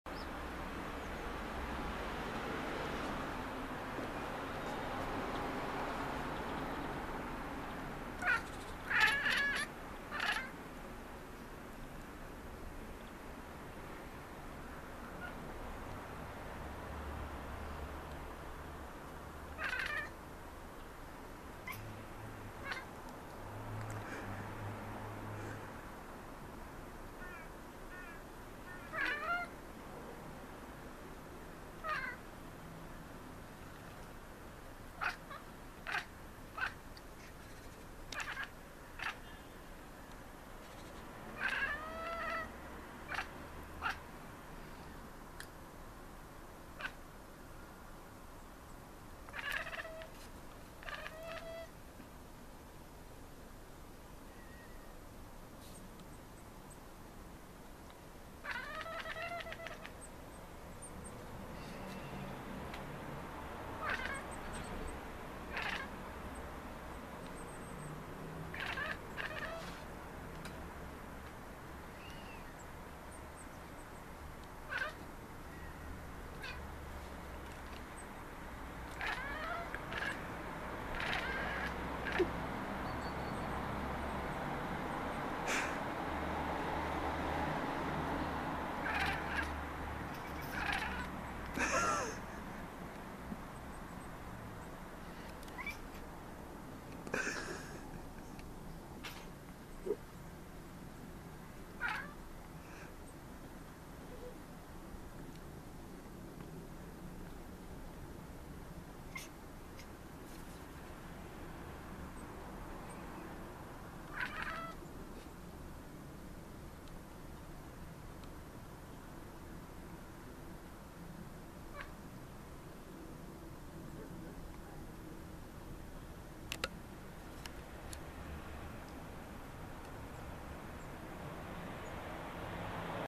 Cute Maine Coons chattering at city birds - pretty funny
Talking to the city´s birds - our 3 super-cute Maine Coon´s favourite amusement.
The main vocalist Flauschy is joined by his red brother Frieso - croaky backing vocals by Lilly.